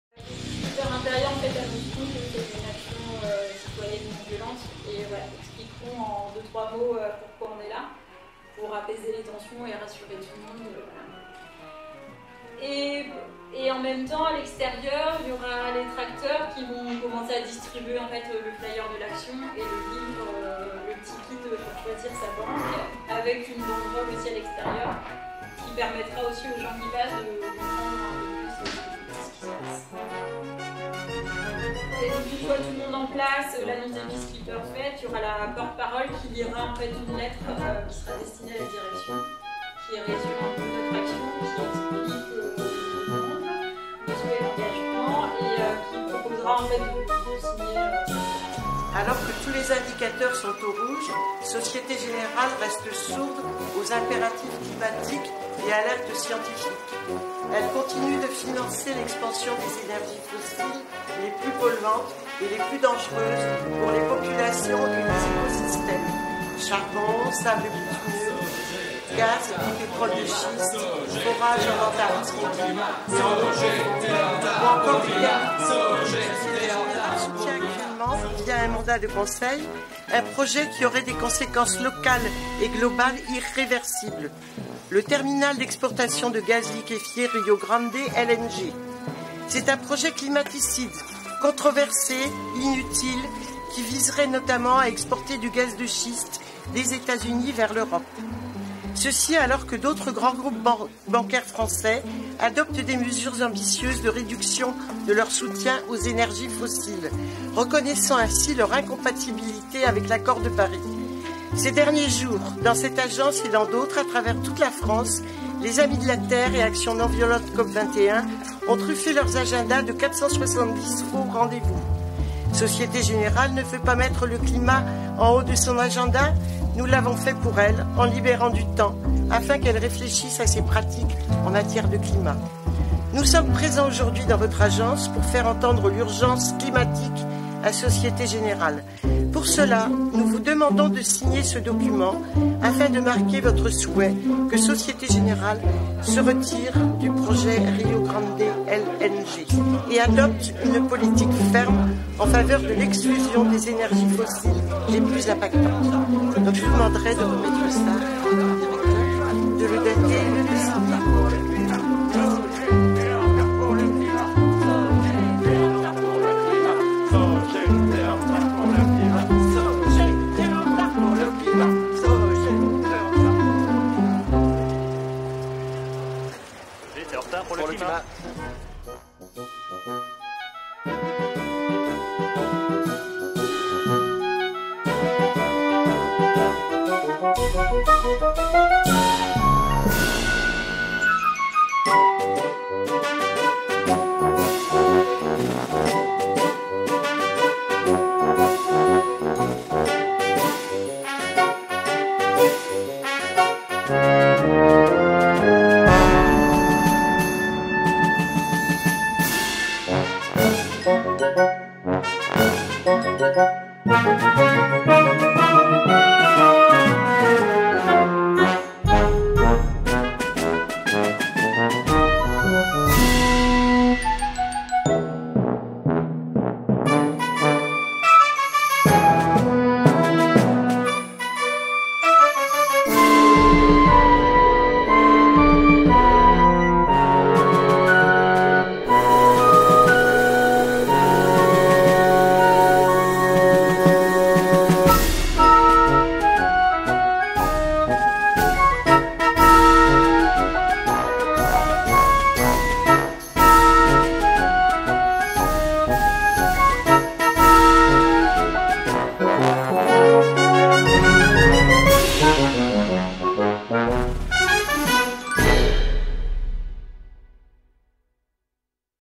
Opération lapins
L'action organisée à Nantes par le Groupement d'Intervention des Grenouilles Non-Violentes et les Amis de la Terre 44